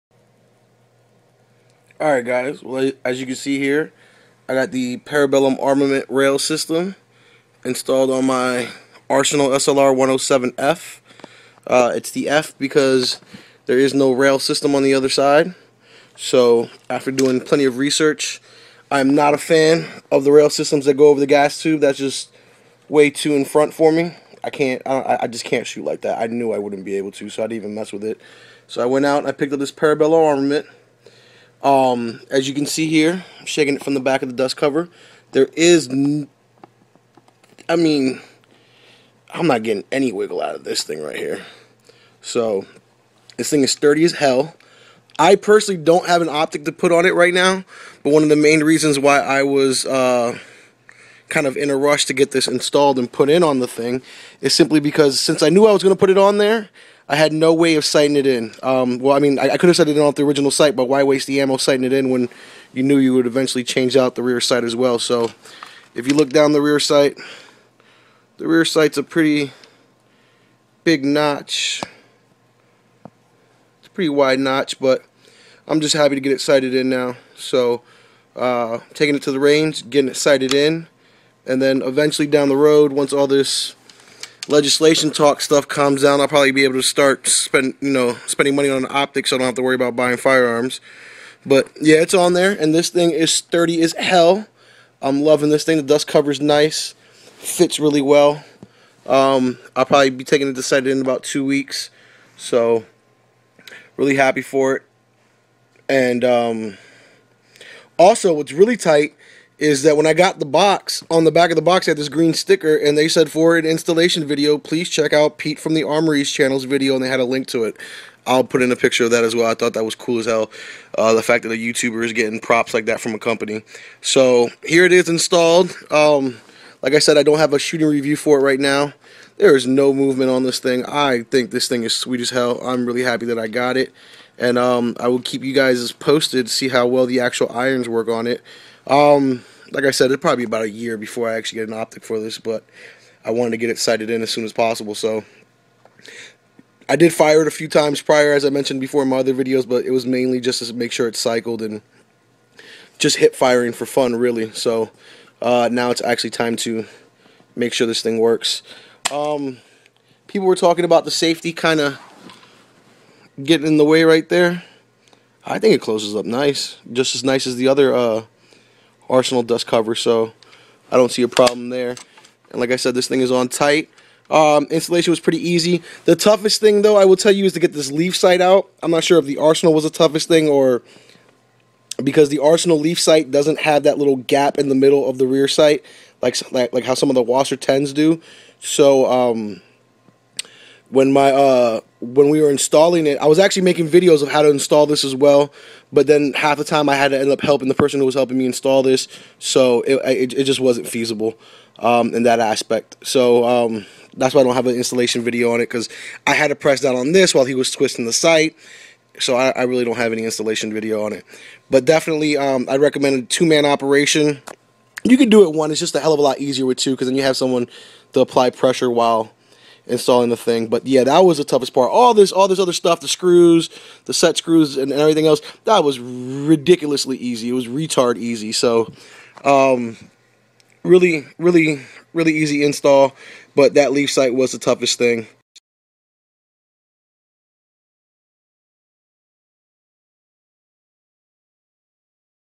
AKRAS ak scope rail Parabellum Armament dust cover rail
Arsenal 107F

how to get a rear mounted scope in an AK 47 74 that has no side scope rail.

runs about $130